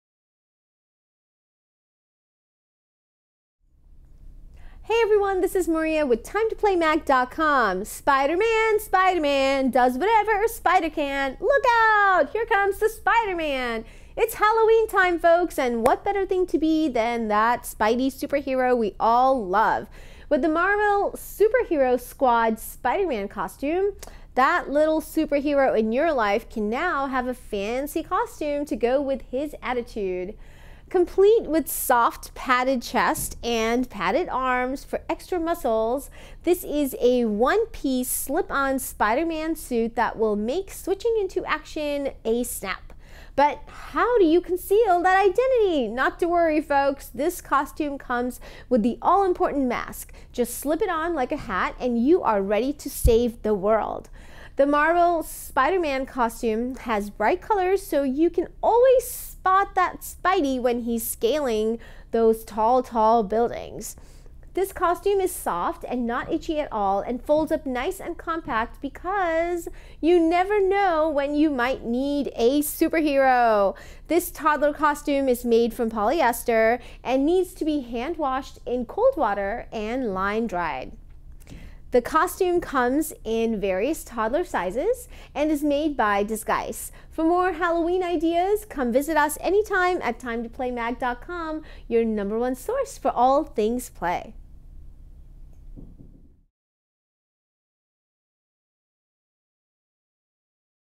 Marvel Superhero Squad Spider-Man Toddler Costume from Disguise
For the full review, where it's in stock, and how much it costs,

With the Marvel Superhero Squad Spider-Man Toddler costume, little superheroes can now dress up as their favorite web-slinging superhero. Complete with a soft padded chest and padded arms for extra bulk, this is a one-piece slip-on Spider-Man suit. The costume also comes with the all-important Spider-Man facemask, which kids can slip on like a hat.